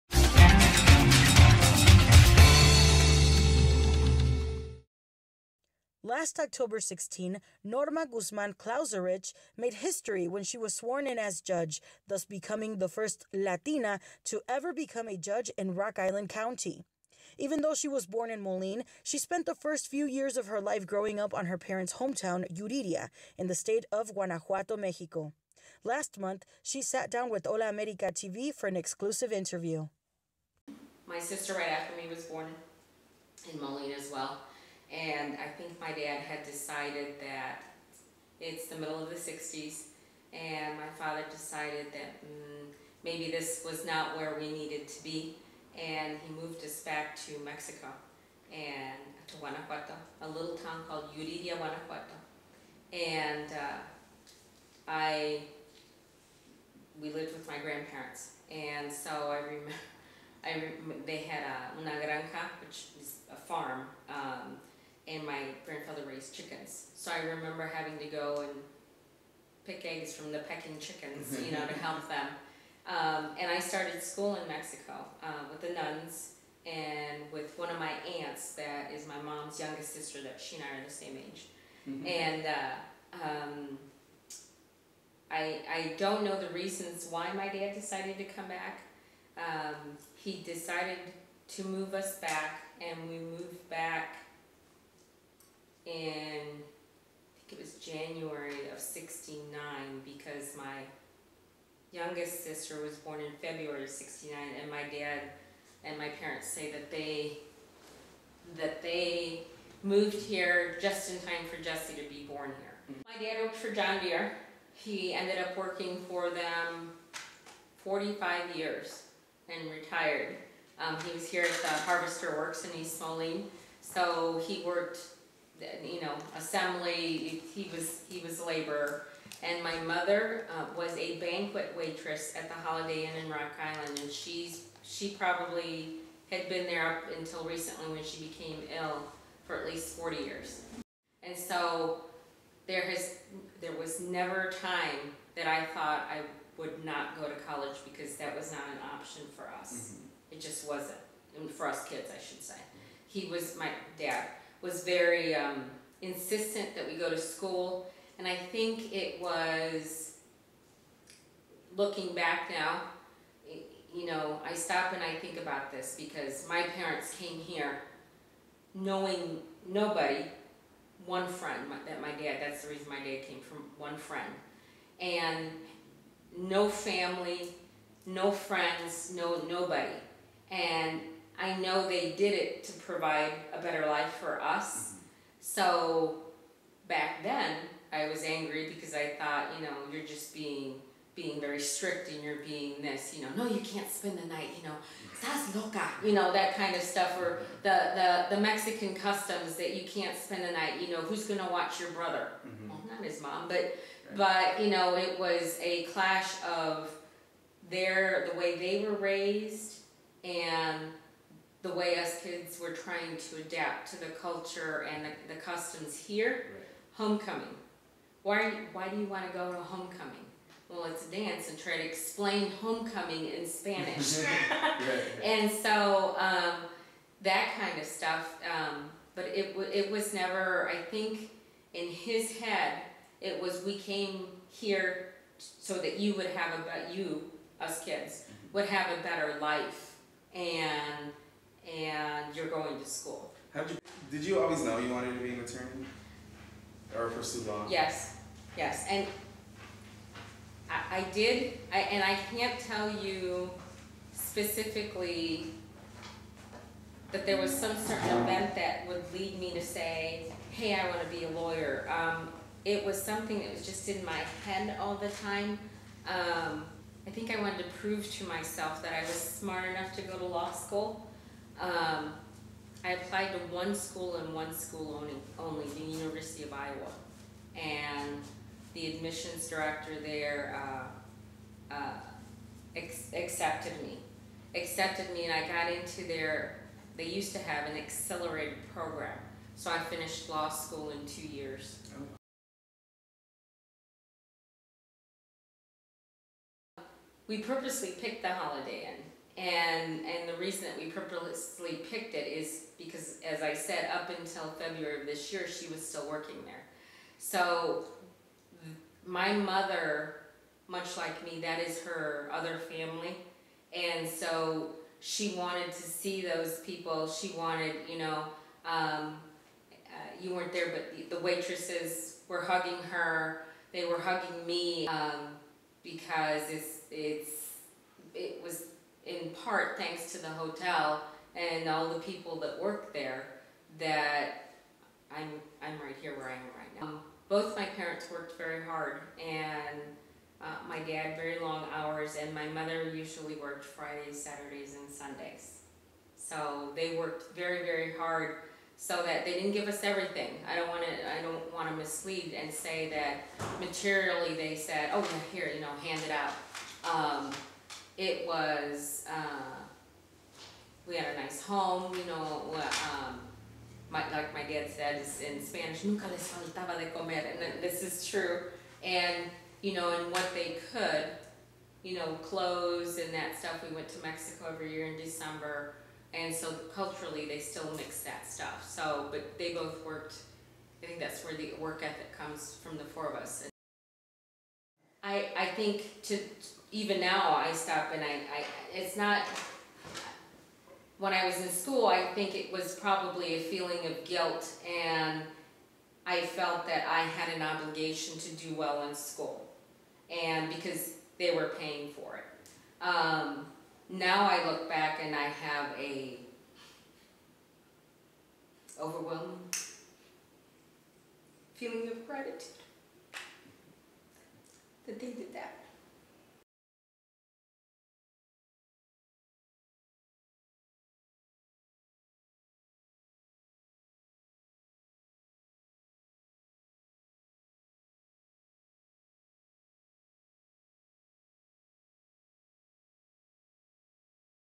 Judge Norma Guzman Kauzlarich the first Latina Judge of Rock Island County
Hola America TV exclusive interview with Judge Norma Guzman Kauzlarich the first Latina Judge of Rock Island County